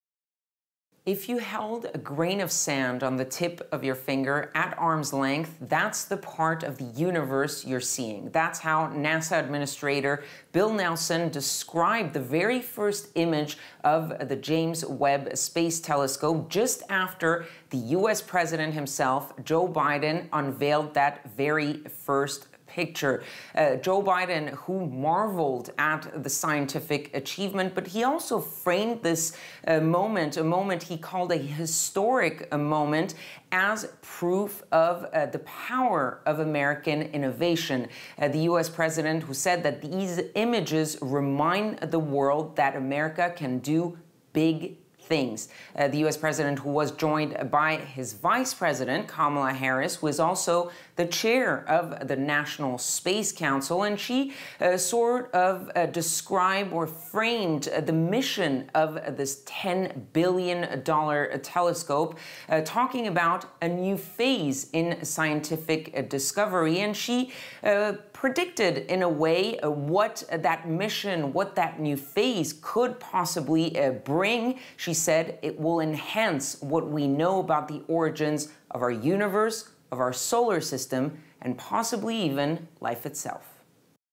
Biden unveils Webb space telescope's first full-color image of distant galaxies • FRANCE 24
The James Webb Space Telescope, the most powerful observatory to be placed in orbit, has revealed the “deepest and sharpest infrared image of the early universe” ever taken, going back 13 billion years, NASA said Monday. FRANCE 24's Kethevane Gorjestani reports.